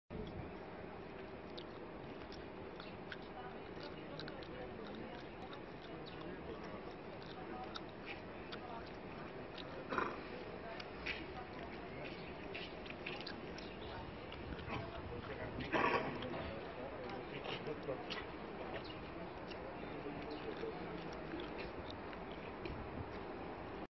Asfalt pri Dome služieb v Dúbravke
Tento asfalt mesto nemôže vymeniť, hoci leží na jeho pozemku. Nebol mu totiž odovzdaný do prevádzky.